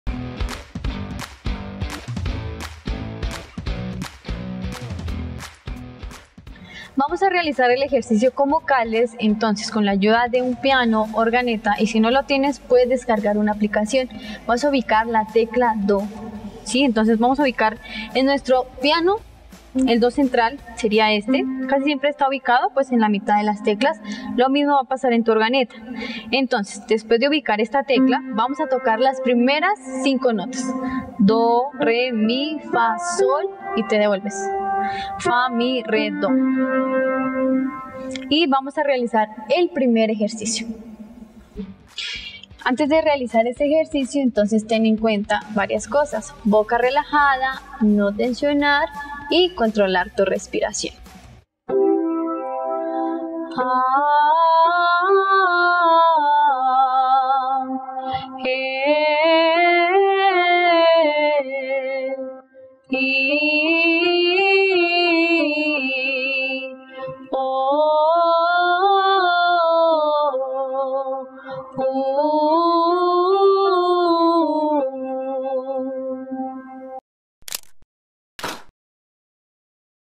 EJERCICIO DE VOCALIZACION #1
Vamos aprender juntos técnicas para el manejo de nuestra voz, alista tus cuerdas vocales y vamos a practicar